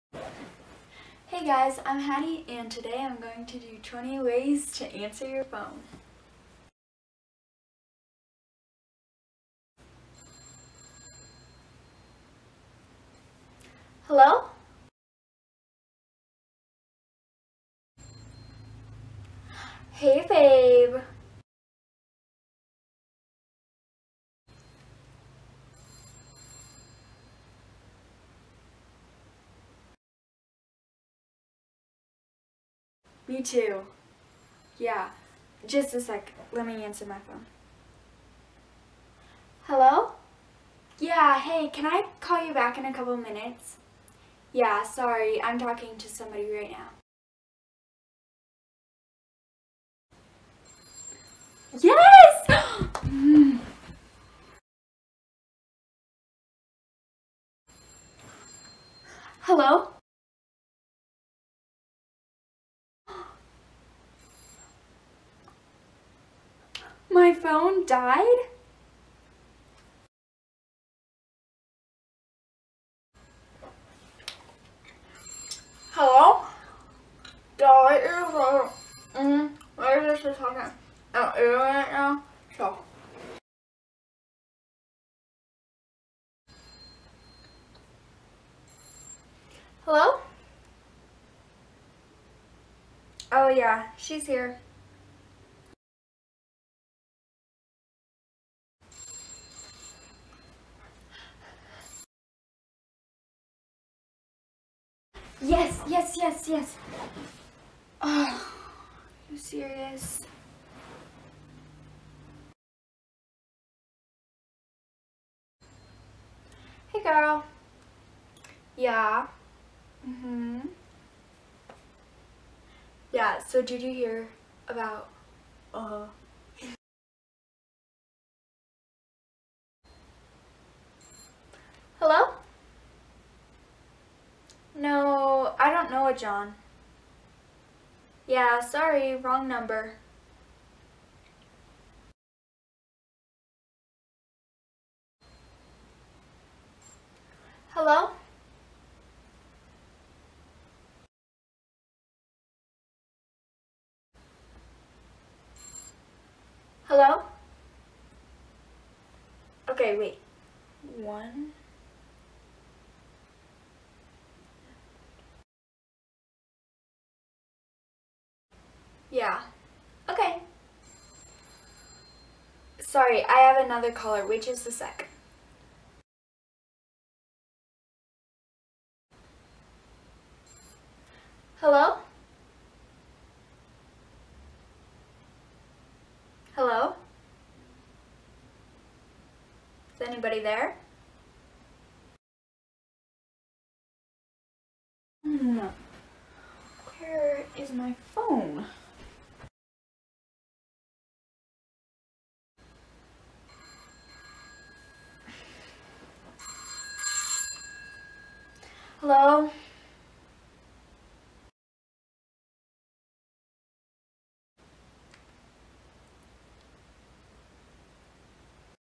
20 Ways to Answer the Phone
Originally posted on September 17, 2018.
Hey, Guys!  There are so many ways to answer your phone.  Here are just a few!